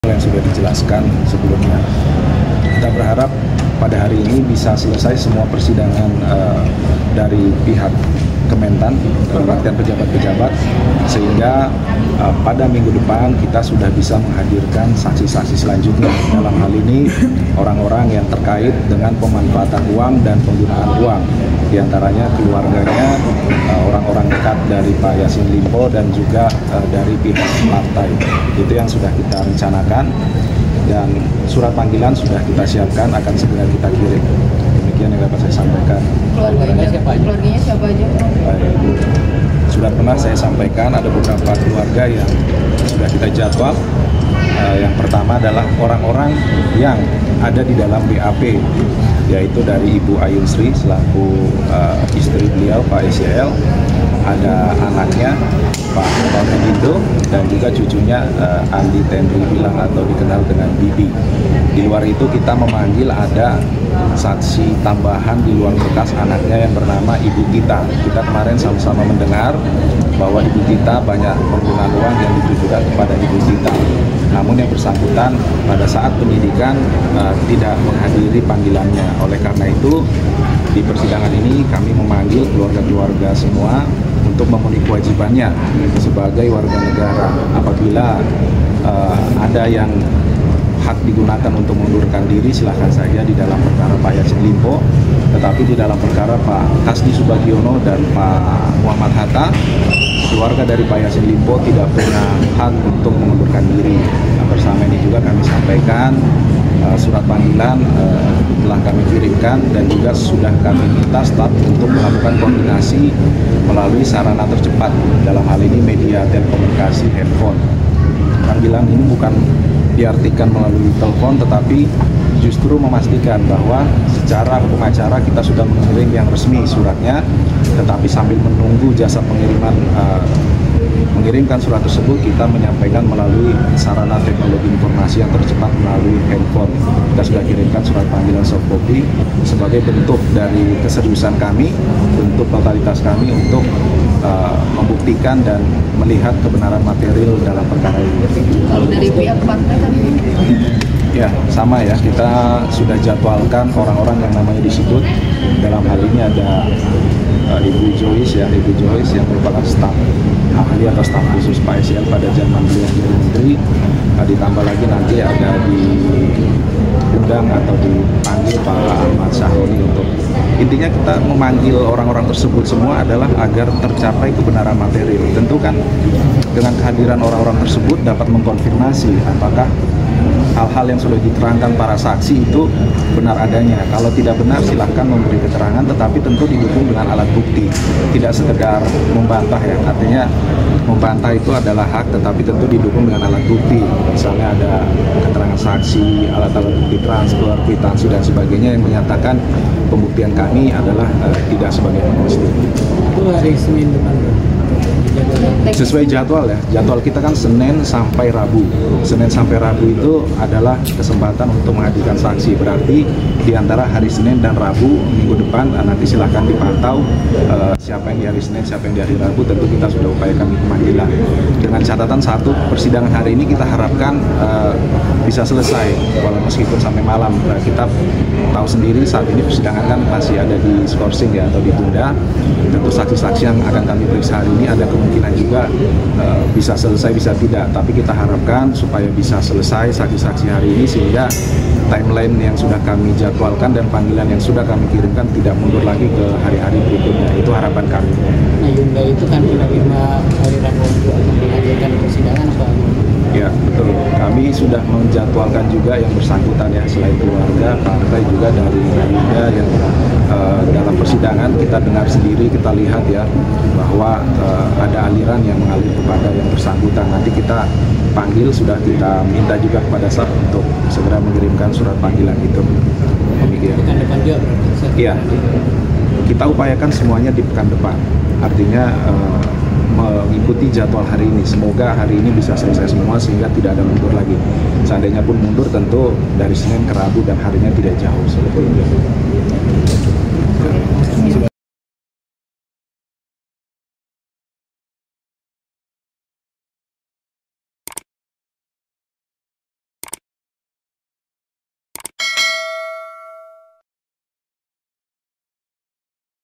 Jaksa KPK Panggil Ahmad Sahroni Jadi Saksi di Sidang SYL
Jaksa Komisi Pemberantasan Korupsi (KPK) Meyer Simanjuntak mengungkapkan, pihaknya berencana untuk menghadirkan orang-orang terdekat dari eks Menteri Pertanian (Mentan) Syahrul Yasin Limpo (SYL), yakni keluarga dan dari pihak partai politik.
"Ibu Ayun Sri selaku istri beliau Pak SYL, ada anaknya Pak Kemal Redindo dan juga cucunya Andi Tendri Bilang atau dikenal dengan Bibi," kata Meyer di Pengadilan Tindak Pidana Korupsi (Tipikor) pada PN Jakarta Pusat, Rabu (22/5/2024).
Selain itu, pihak Jaksa KPK juga akan memanggil saksi tambahan, yakni anak SYL Indira Chunda Thita Syahrul.
Sementara, dari partai politik terkait dengan SYL, ada nama Bendahara Umum Partai Nasdem Ahmad Sahroni.
"Ada Ibu Joice yang merupakan staf ahli atau staf khusus Pak SYL pada jaman dia menjadi menteri. Ditambah lagi nanti ada diundang atau dipanggil Pak Ahmad Sahroni," papar Meyer.
Video Jurnalis: Talitha YumnaaPenulis Naskah: Talitha YumnaaVideo Editor: Talitha YumnaaProduser: Adil Pradipta